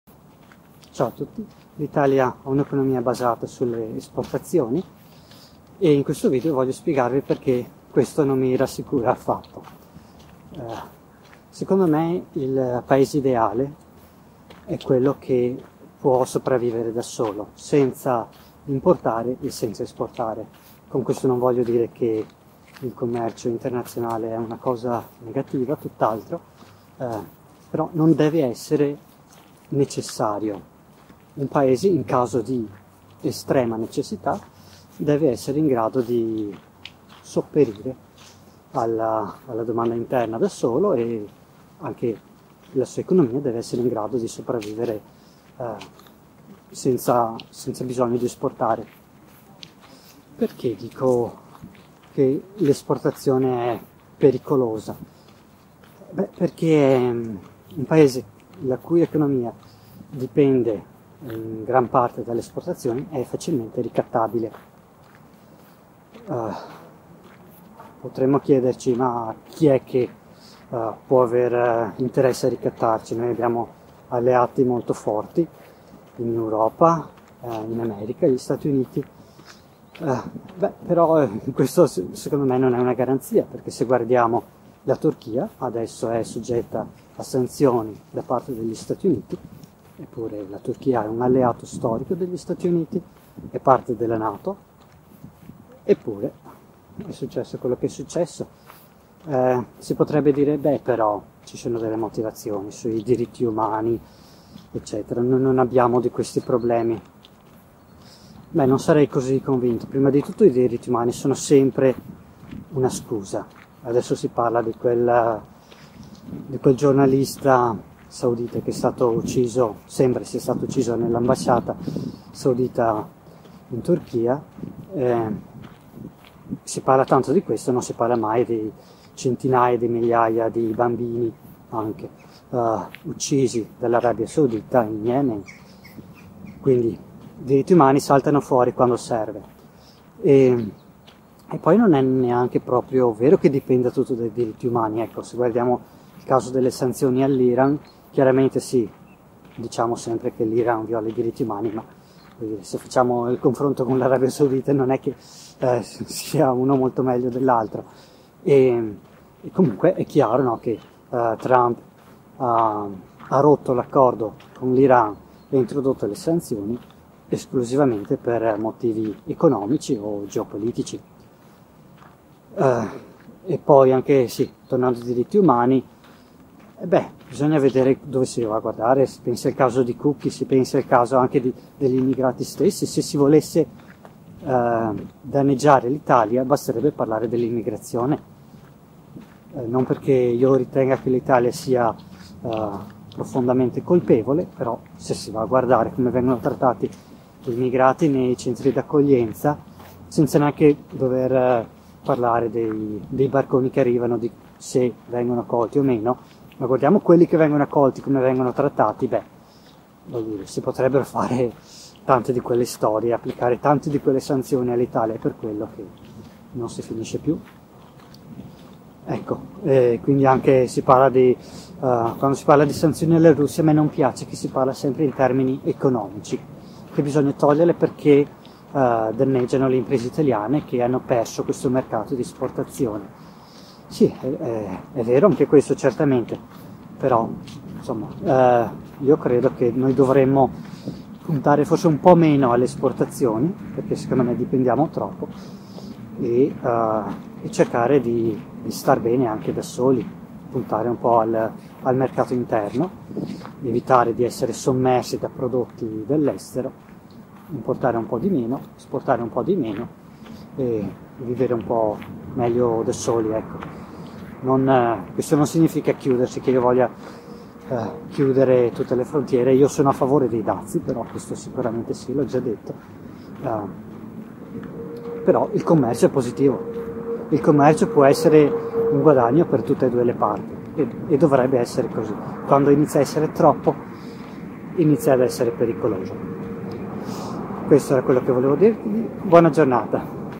Cammino quindi penso - 2018-10-17 - Puntiamo sulle esportazioni!
Gli economisti considereranno blasfema la mia opinione, ma siccome noi matematici siamo un po' slegati dalla realtà, sono fiducioso nel vostro perdono. In breve: un paese la cui economia dipende dalle esportazioni è un paese ricattabile.